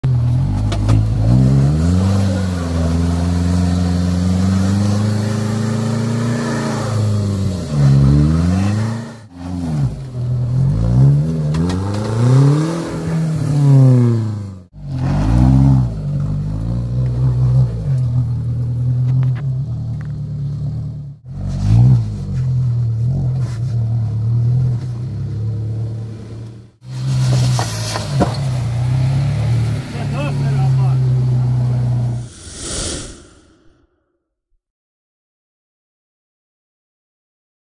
Na trilha off road 4x4: Renault Duster 2.0 4wd Pneus AT
Quem tem dó é violão! Duster 4x4 na trilha com valas e pedras.